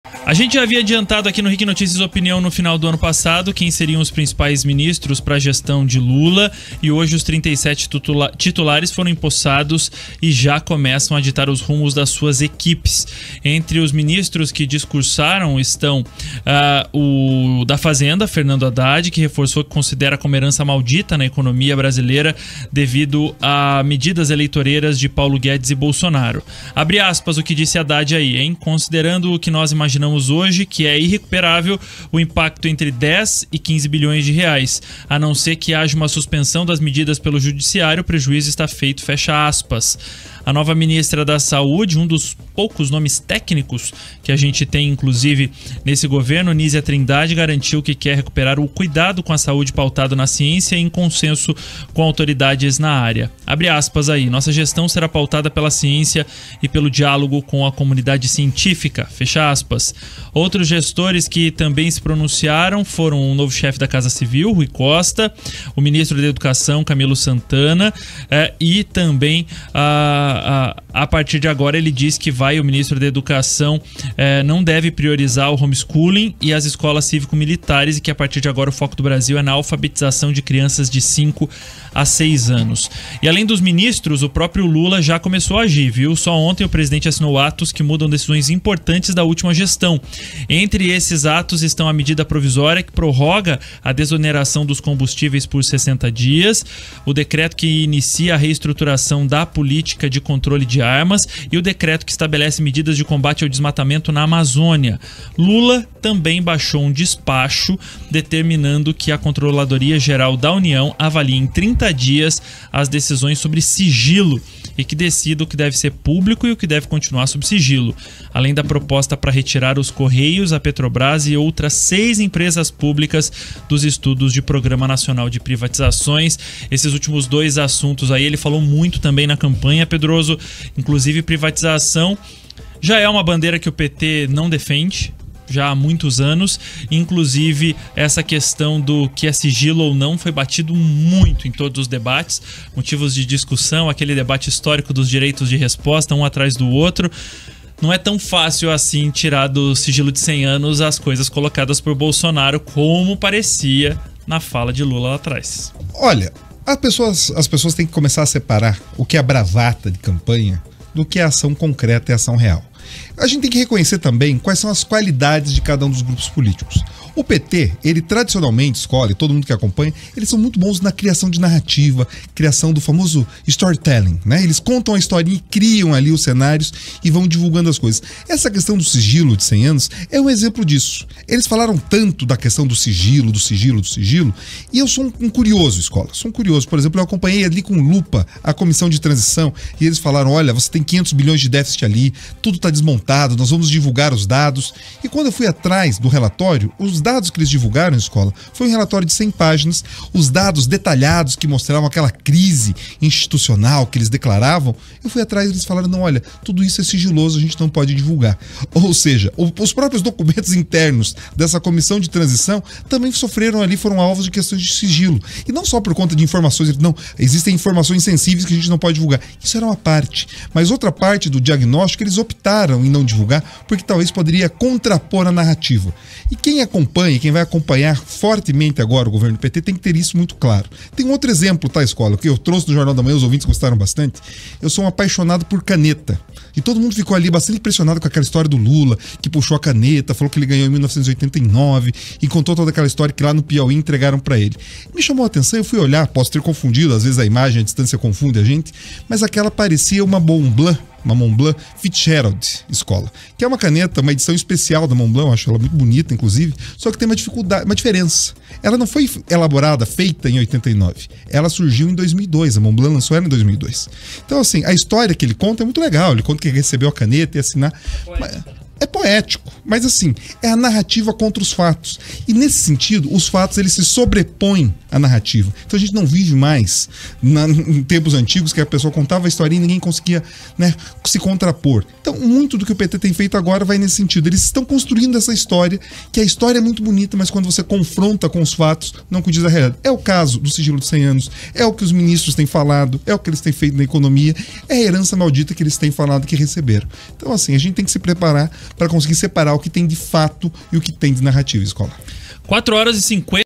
Novos ministros criticam a “herança maldita” de Bolsonaro
Entre os ministros que discursaram hoje está o titular da Fazenda, Fernando Haddad, que reforçou o que ele considera como herança maldita na economia brasileira devido a medidas eleitoreiras de Paulo Guedes e Bolsonaro.